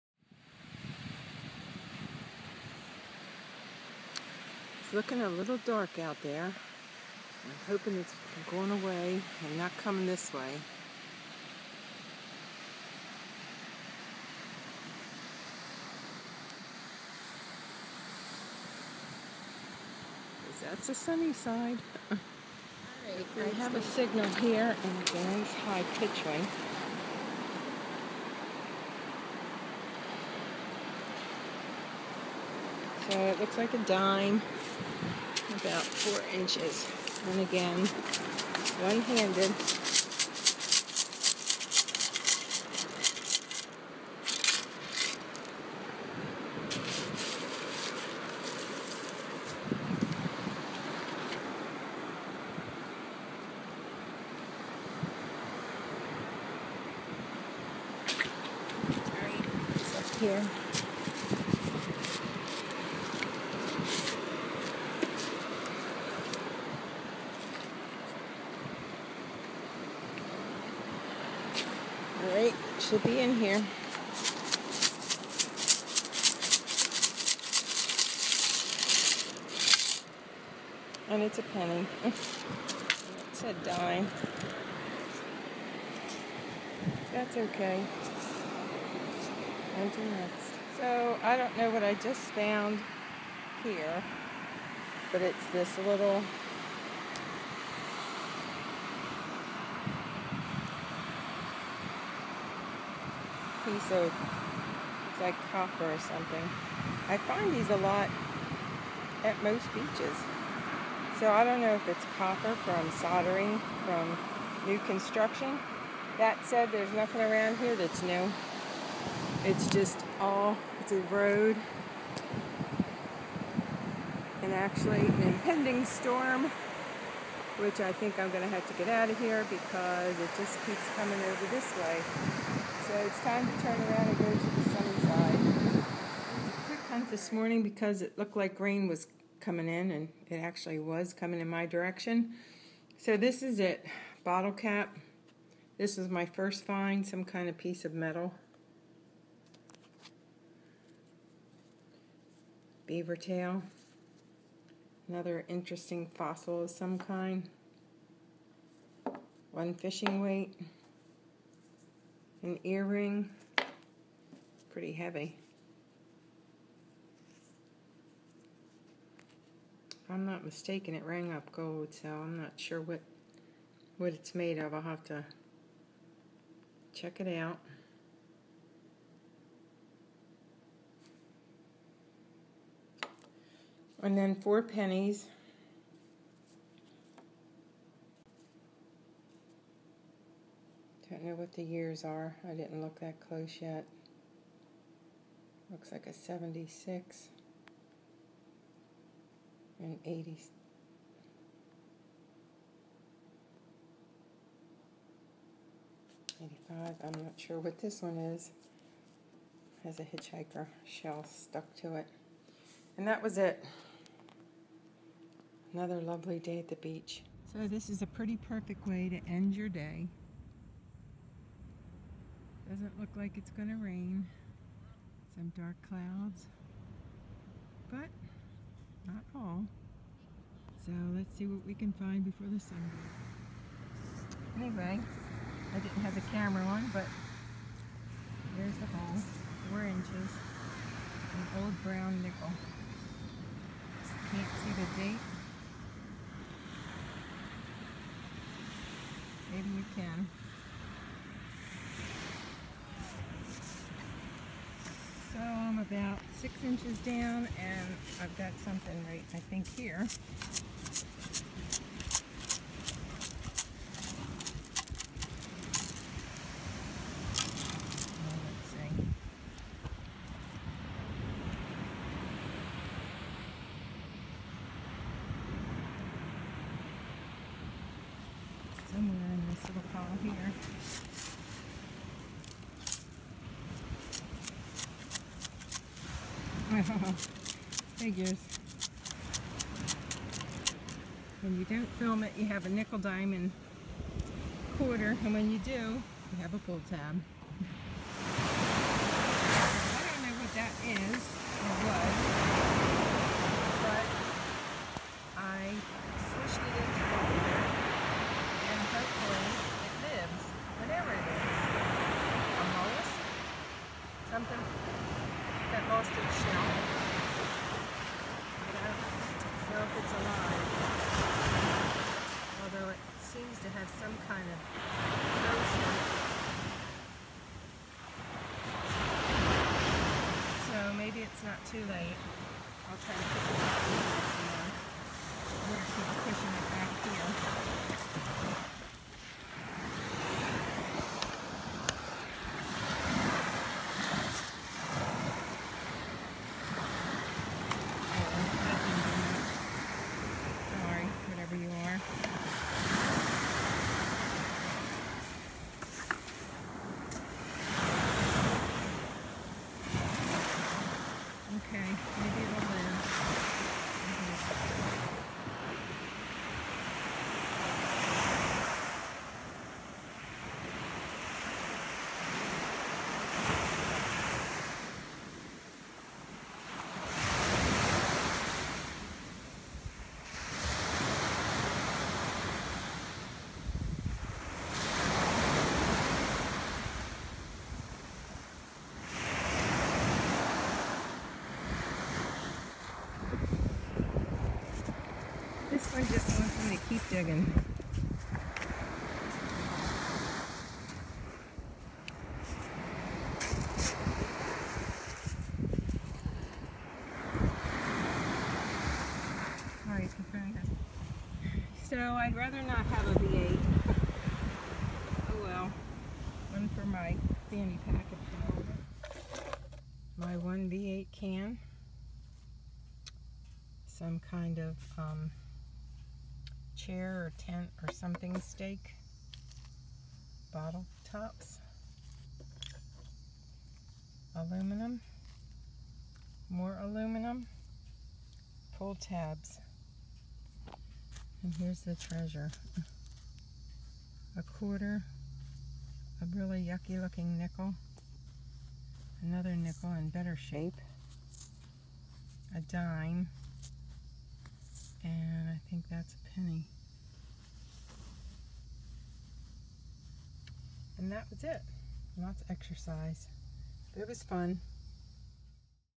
06 10 2016 Morning and Evening Metal Detecting in SW Florida
Morning hunt was rainy, but evening hunt was perfect!